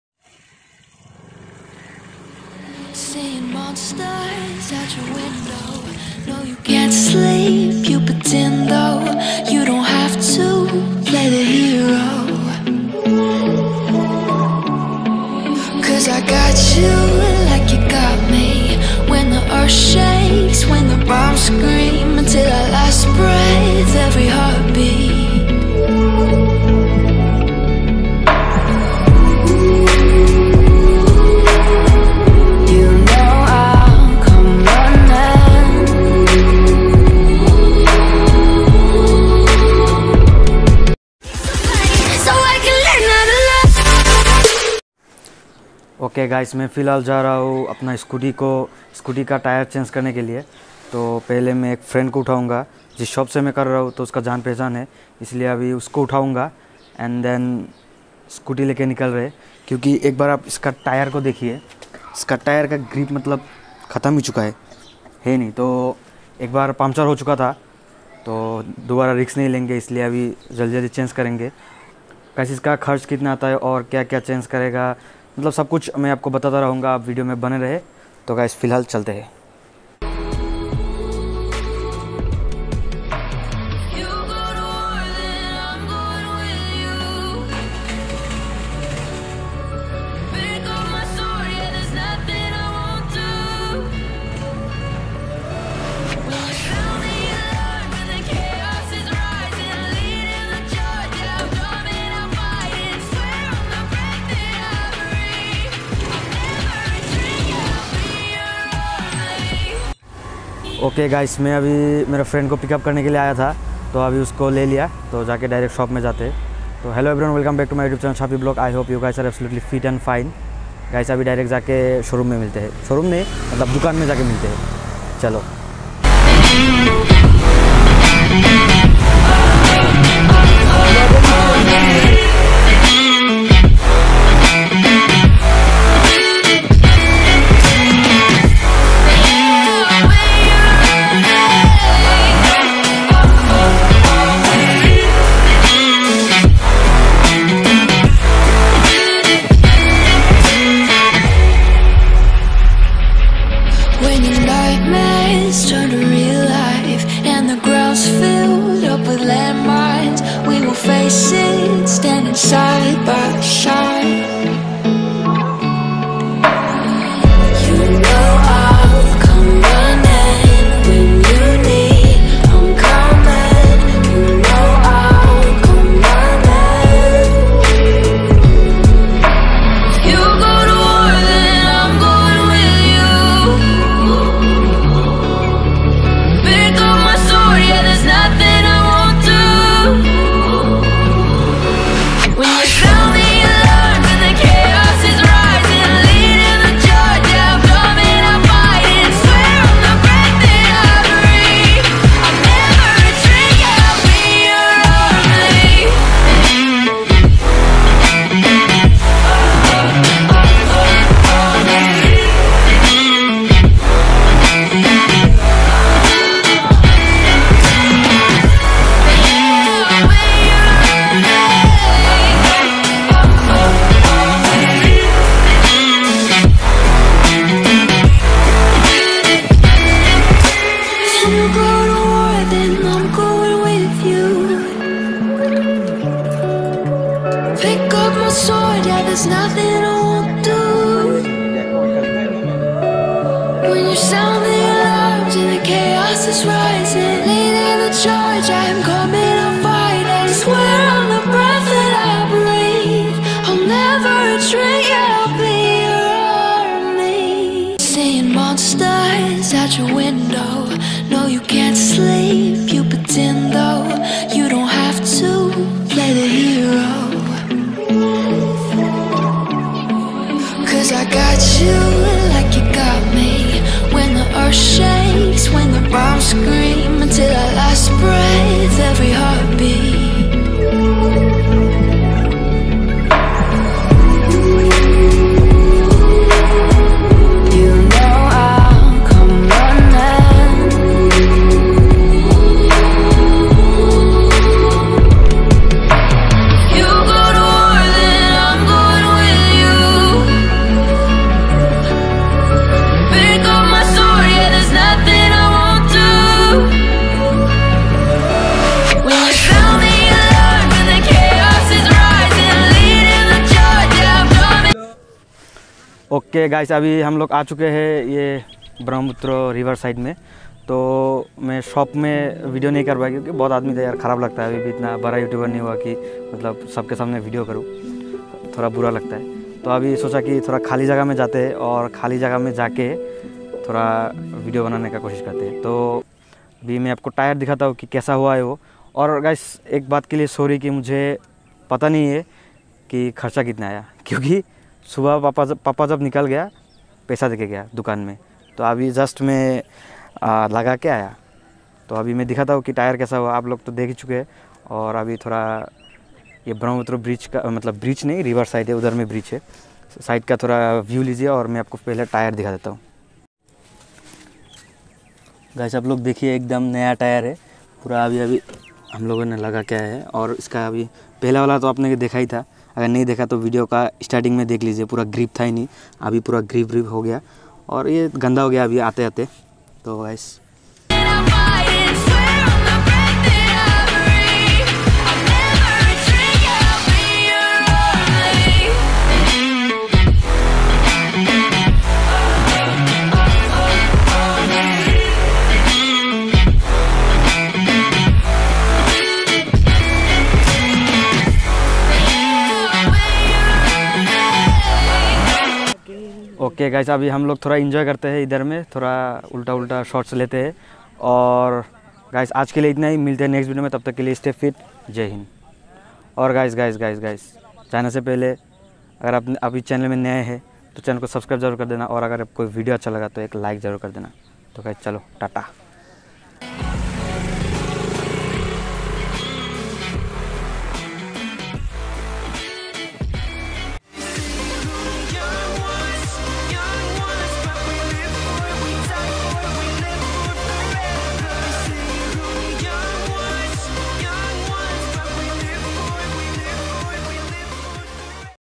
Scooty Tire Change | New Tire | Hero Maestro Edge | Shafi Vlog
THANK YOU. !